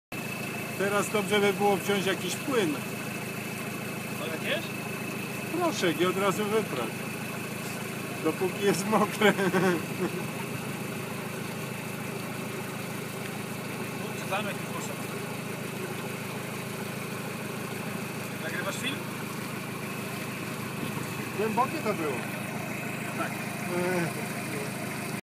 Mycie auta na poligonie w okolicach Słupska
Po przejażdżce przez kałuże okazało się, że część kałuż koleżanka zabrała ze sobą. Więcej filmów na